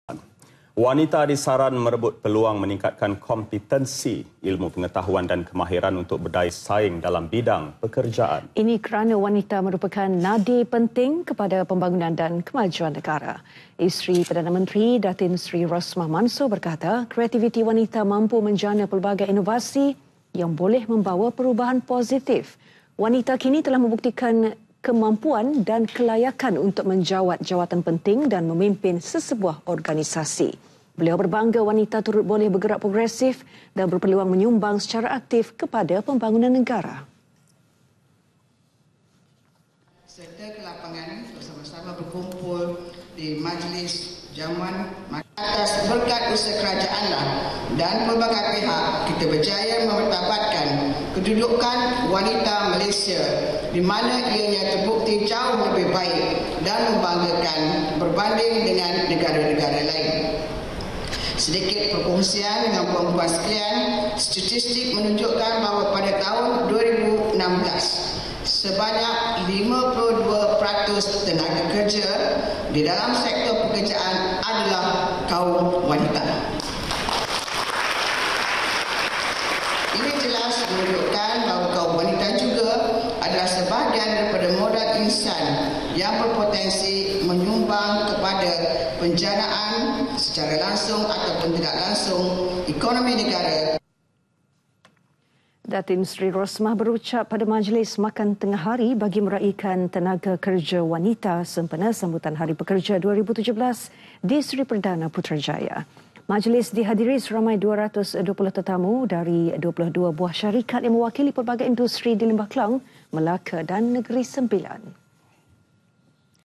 WANITA – KEKAL NADI PENTING PEMBANGUNAN NEGARA [2 MEI 2017]
Wanita disaran merebut peluang meningkatkan kompetensi, ilmu pengetahuan dan kemahiran untuk berdaya saing dalam bidang pekerjaan.